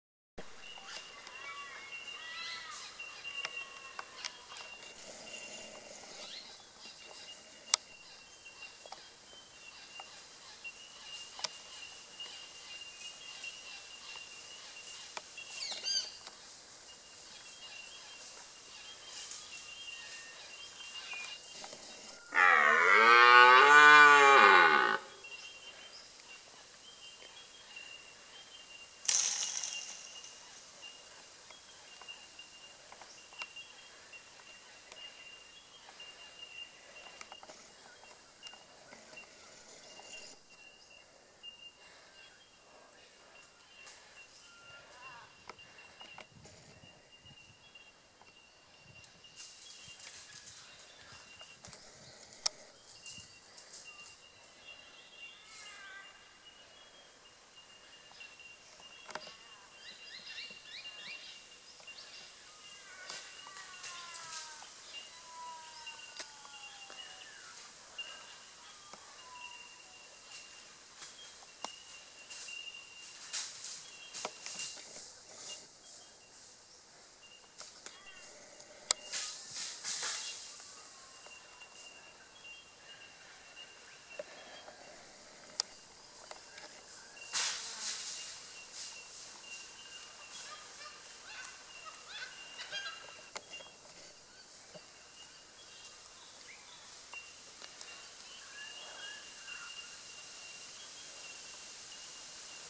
young red stag roaring
had this red stag walk up to me at under 7m over the weekend.